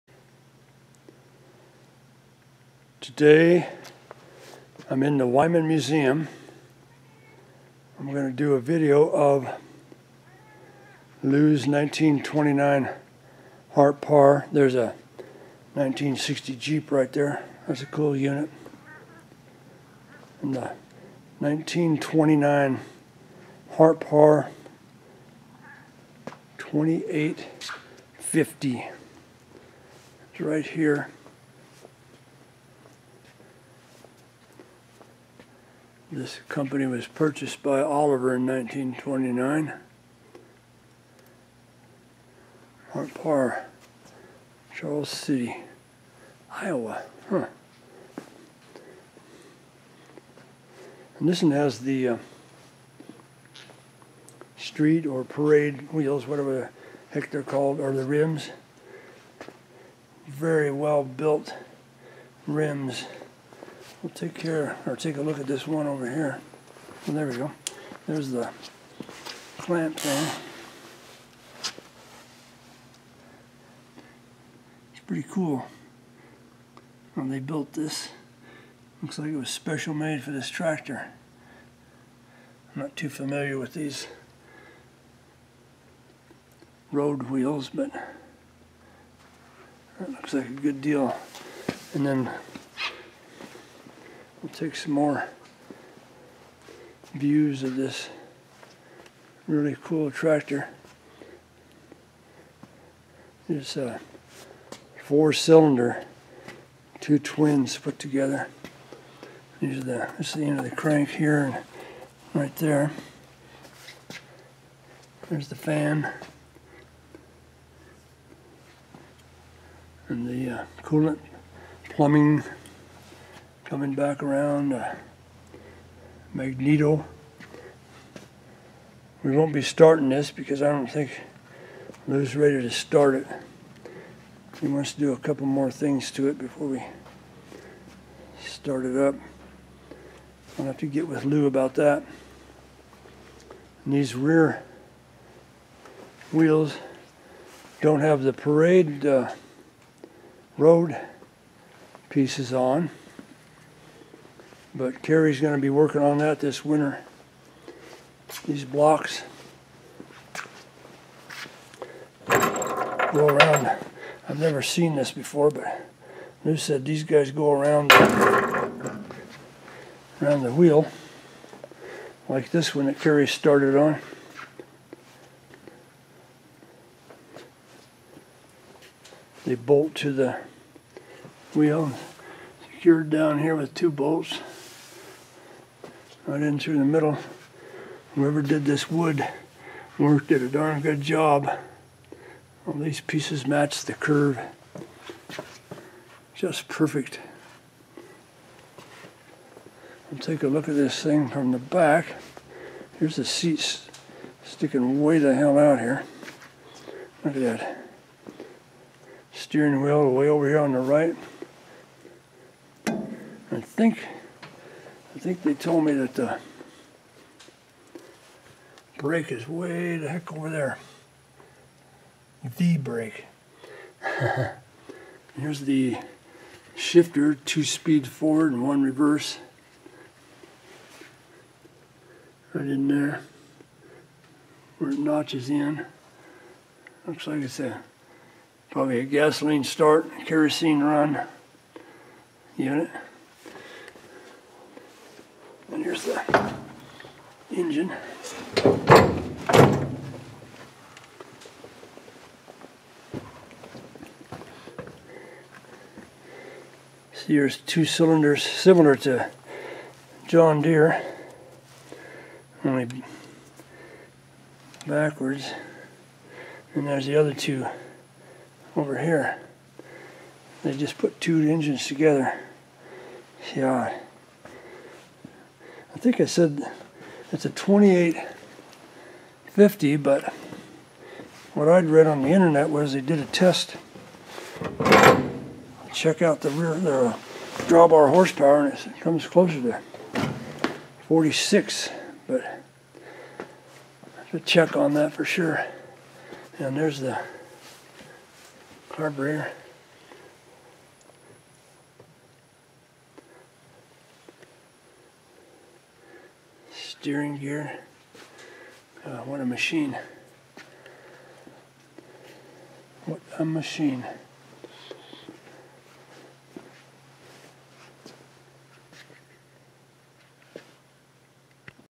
1929 Hart Parr 28-50 tractor.
1929 Hart Parr 28-50 tractor. The Wyman Museum recently acquired this nice tractor.